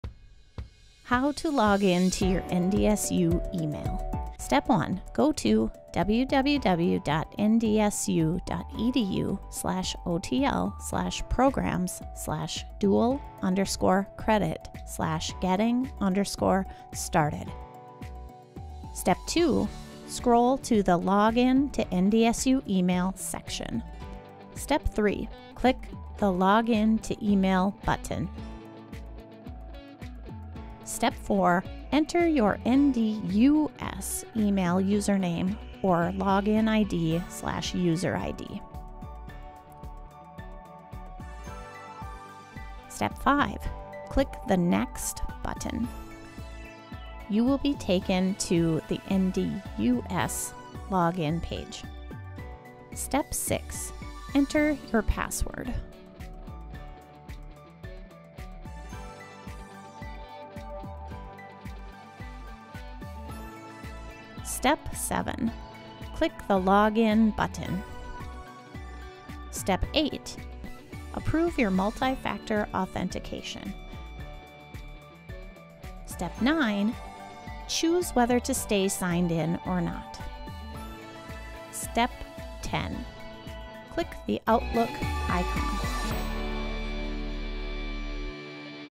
How to Login to Your NDSU Email
Walk through the process of finding and logging in to your NDSU email account.